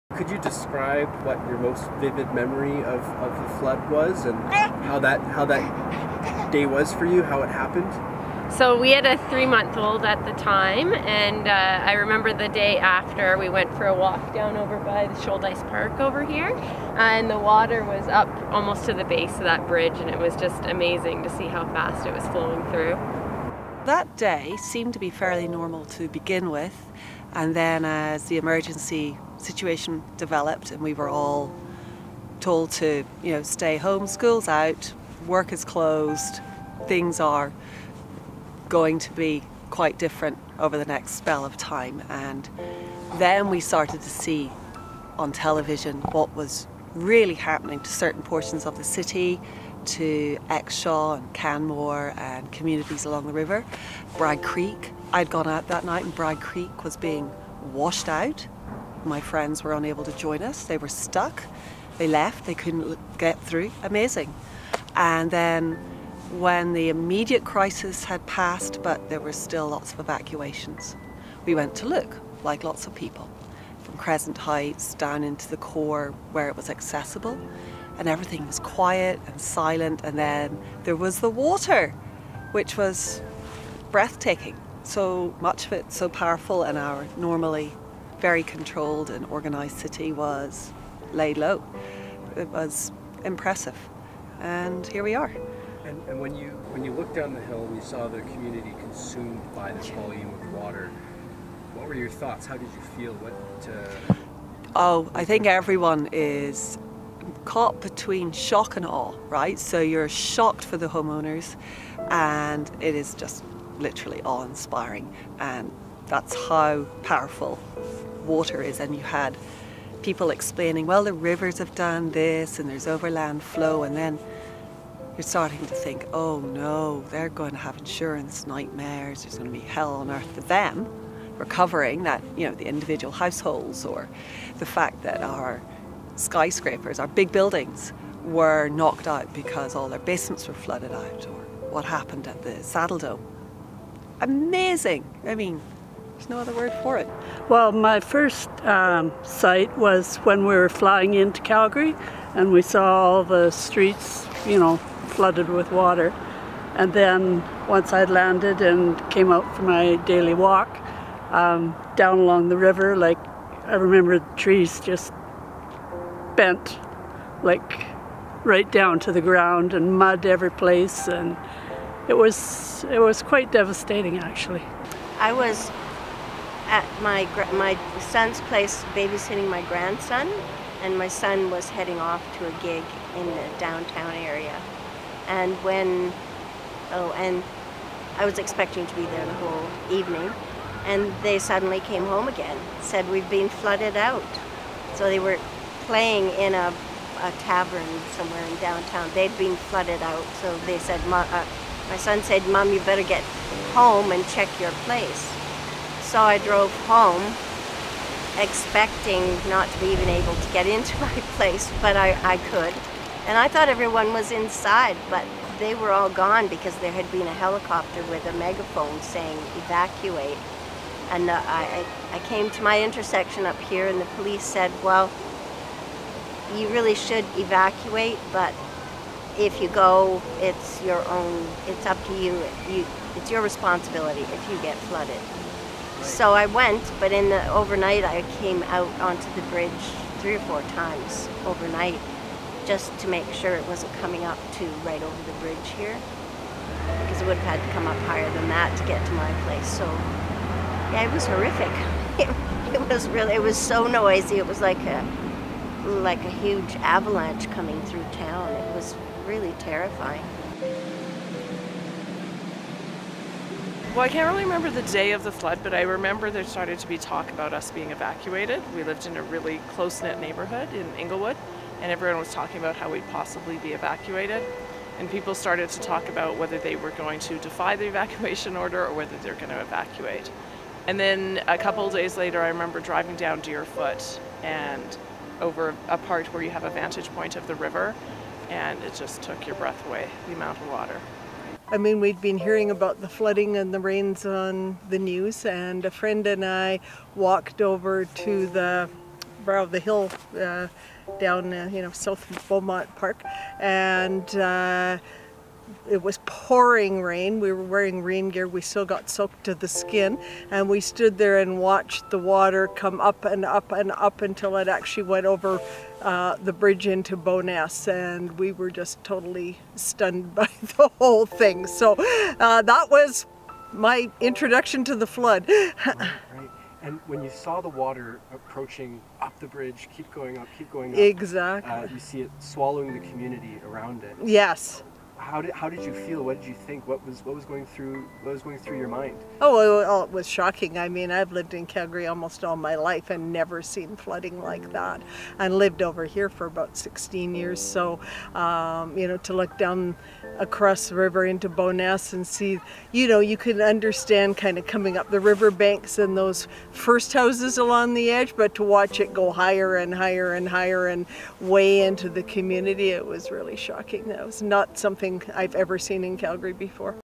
Looking back at the Calgary flood
In 2013, Calgary experienced one of the most destructive floods in history. 6 billion dollars in damages were recorded and thousands of people were evacuated because of it. In this video, we see the perspectives of some of those who witnessed the floods and what they experienced back in 2013.

By: Alaina Shirt, Gabriel Reed, Matt Hull